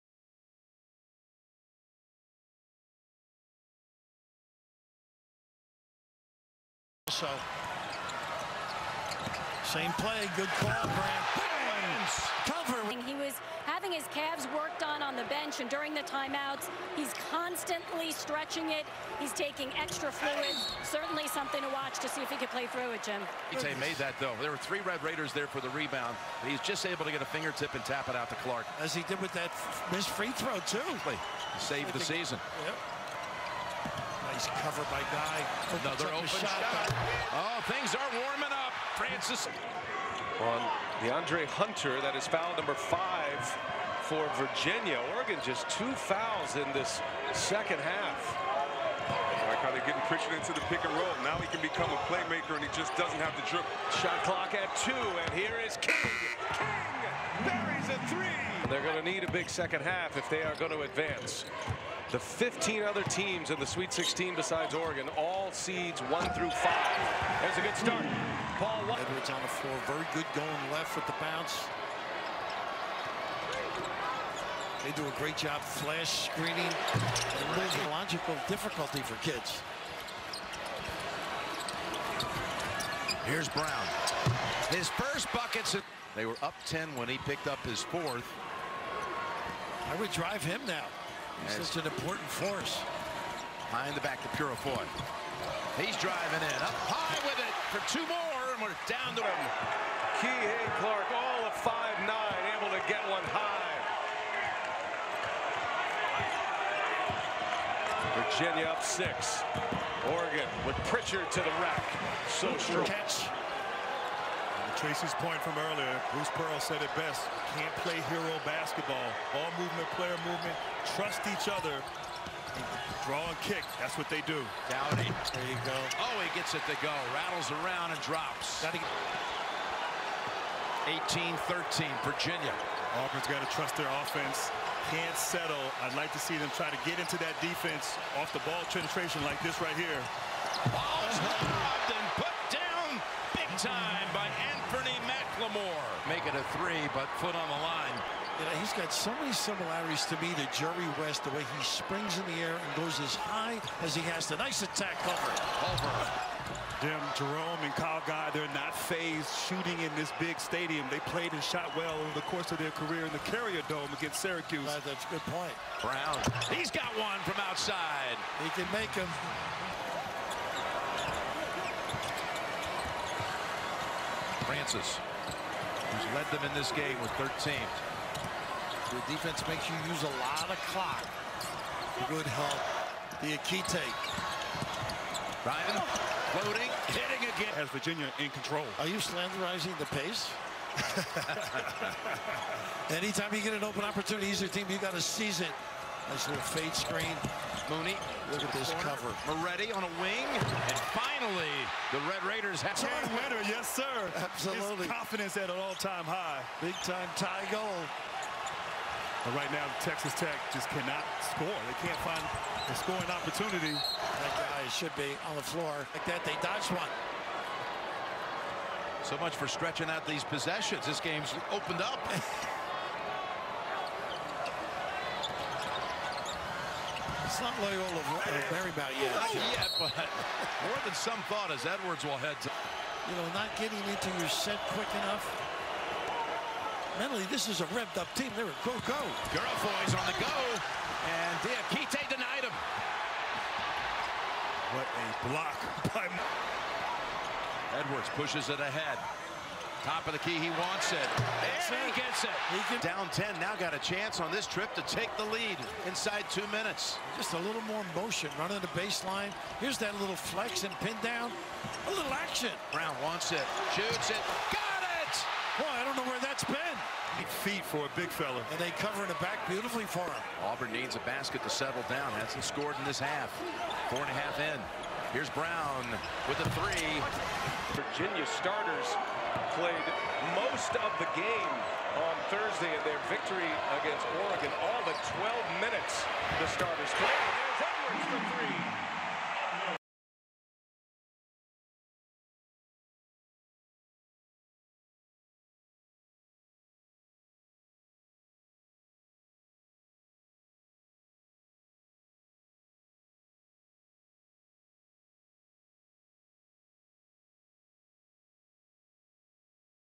How to beat the Pack Line defense
This video shows actions that teams used to beat Virginia's Pack Line defense in the 2019 NCAA tournament